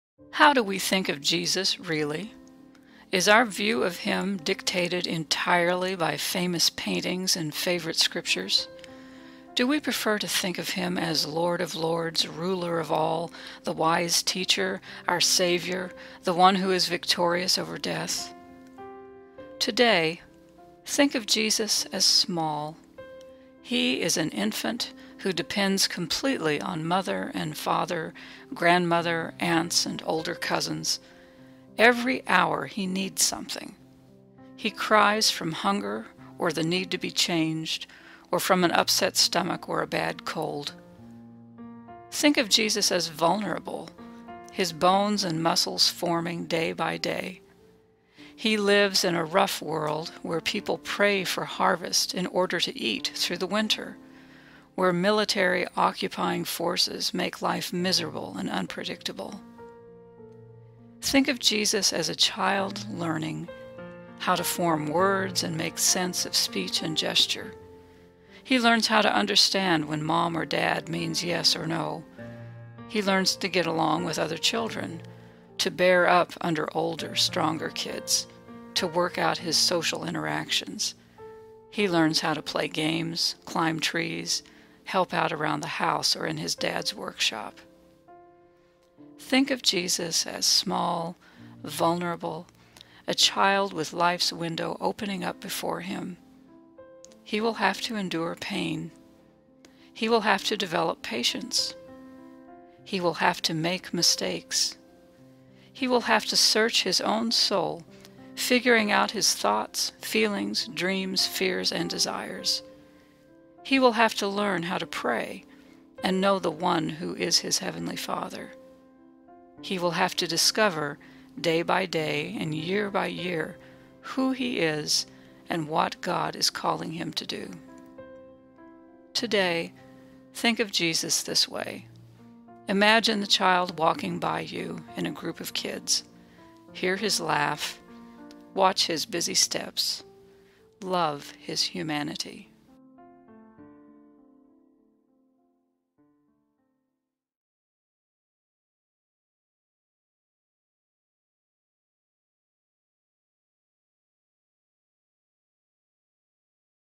Jesus the Child Reflection by Vinita Hampton Wright
How do we think of Jesus, really? Vinita Hampton Wright reflects and invites you to imagine the child Jesus.